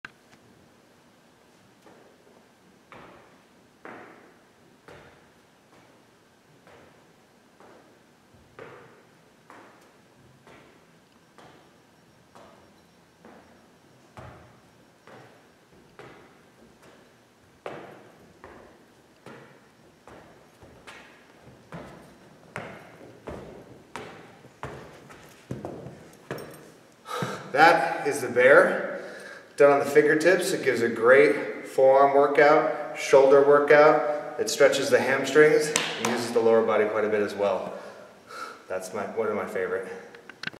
The Bear
This warmup incorporates hamstrings, glutes, and upper body for a quick, effective way to get your body ready for weight training work outs.